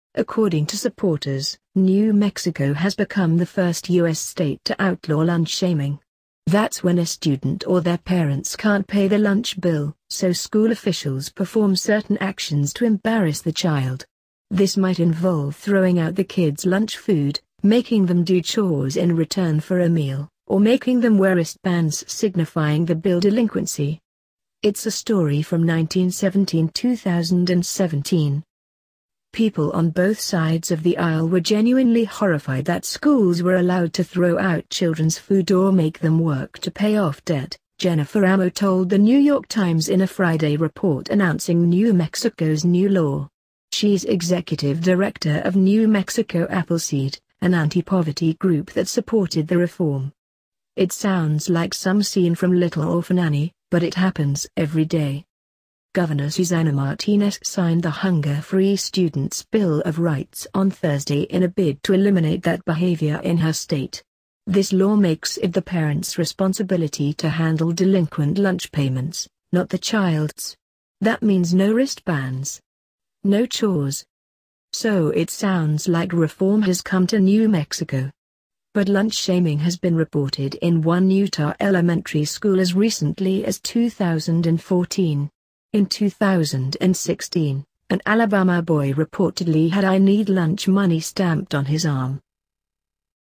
New Law Stops Schools from Making Students Do Chores for Lunch
According to supporters, New Mexico has become the first U.S. state to outlaw “lunch shaming.” That’s when a student or their parents can’t pay the lunch bill, so school officials perform certain actions to embarrass the child. This might involve throwing out the kid’s lunch food, making them do chores in return for a meal, or making them wear wristbands signifying the bill delinquency. It’s a story from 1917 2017.

“People on both sides of the aisle were genuinely horrified that schools were allowed to throw out children’s food or make them work to pay off debt,” Jennifer Ramo told The New York Times in a Friday report announcing New Mexico’s new law. She’s executive director of New Mexico Appleseed, an anti-poverty group that supported the reform. “It sounds like some scene from ‘Little Orphan Annie,’ but it happens every day.”

Governor Susana Martinez signed the Hunger-Free Students’ Bill of Rights on Thursday in a bid to eliminate that behavior in her state. This law makes it the parents’ responsibility to handle delinquent lunch payments, not the child’s. That means no wrist bands. No chores.

So it sounds like reform has come to New Mexico. But “lunch shaming” has been reported in one Utah elementary school as recently as 2014. In 2016, an Alabama boy reportedly had “I need lunch money” stamped on his arm.